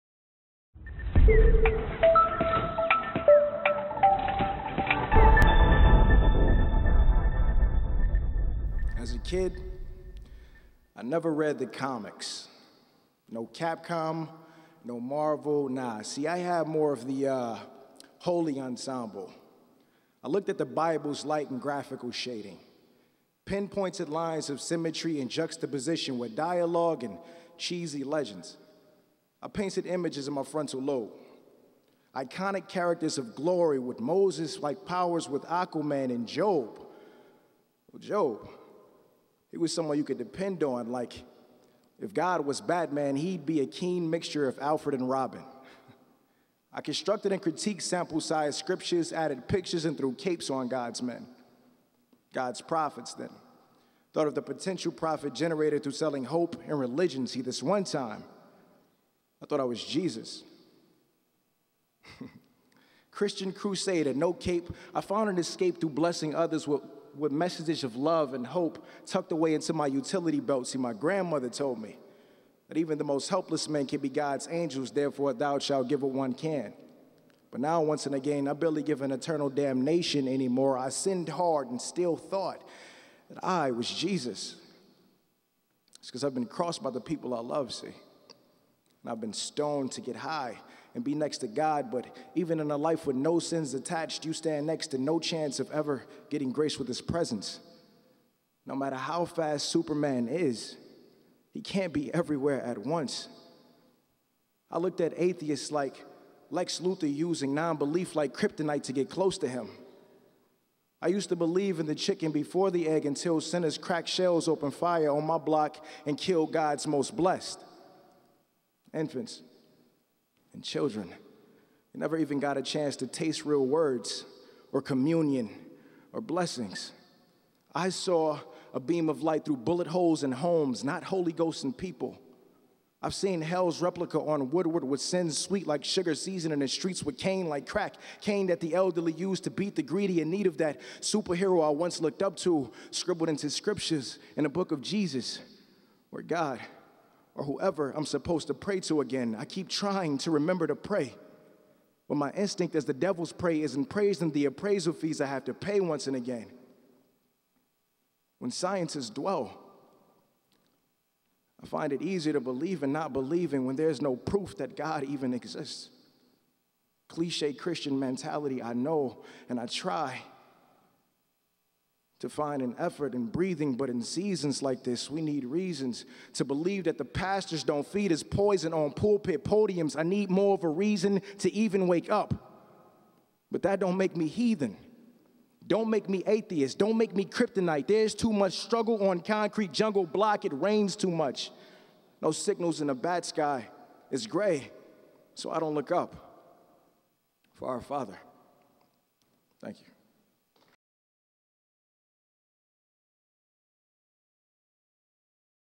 NavigatingGOD: Terrell Morrow at TEDxDetroit 2013
Terrell Morrow is a  2013 graduate of Mumford High School where he participated in InsideOut's award-winning after-school program, Citywide Poets.  Most recently, Terrell was part of InsideOut's 2013 slam team representing Detroit at the Brave New Voices Youth Poetry Festival in Chicago, IL.